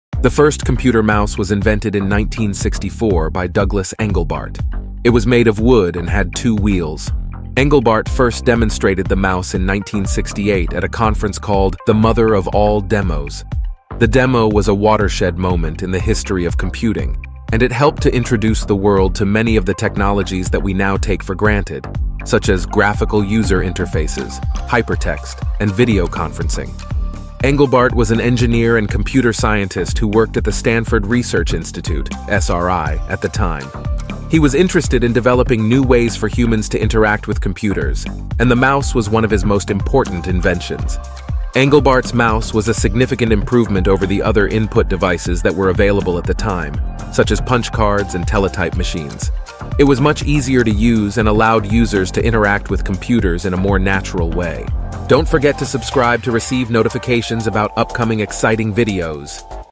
The Birth of the Computer Mouse: Douglas Engelbart's Game-Changing Invention
The first computer mouse was invented in 1964 by Douglas Engelbart. It was made of wood and had two wheels.